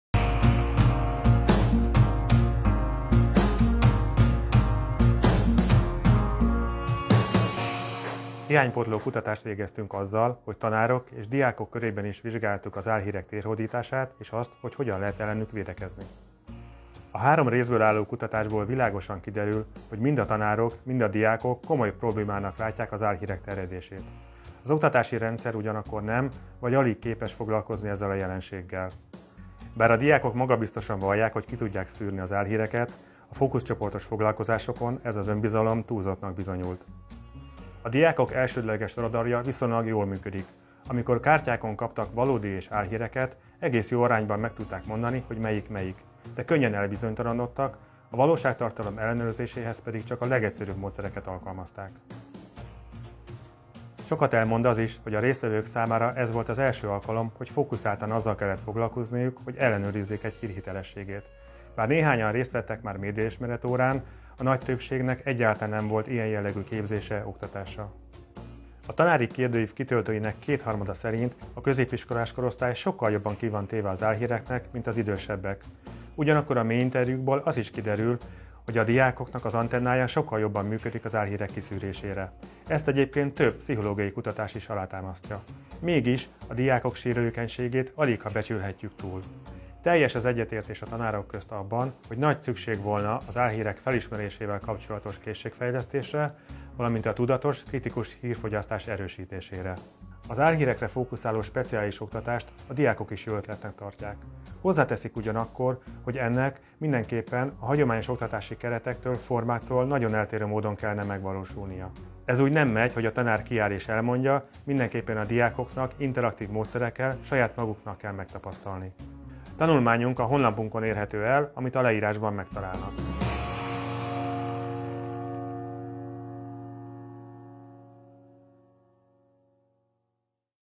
Álhírek elleni küzdelem az oktatásban
A koronavírus, illetve annak terjedése kiválóan mutatja az álhírek veszélyességét. Hogy látják az álhírek problémáját a diákok és a tanárok?
Noha sok szó esik arról, hogyan vehetjük fel a harcot az álhírekkel, és ebben sokan központi szerepet szánnak az iskolának, szinte semmit nem tudunk arról, miként látják a kérdést maguk a tanárok és a fiatalok. 🔎 E hiány pótlására kutatást végeztünk tavaly év végén a középiskolai tanárok és fiatalok körében. Ennek során a kérdőívünket kitöltő tanárok kétharmada úgy vélte, hogy a középiskolás korosztály a felnőttekhez képest jobban ki van téve az álhíreknek.
📊 Az álhírek problémáját mind a diákok, mind a tanárok jelentősnek látják, az oktatási rendszer ugyanakkor kevésbé képes kezelni ezt a jelenséget főként a pedagógusok leterheltsége miatt.
További részletek a videóban.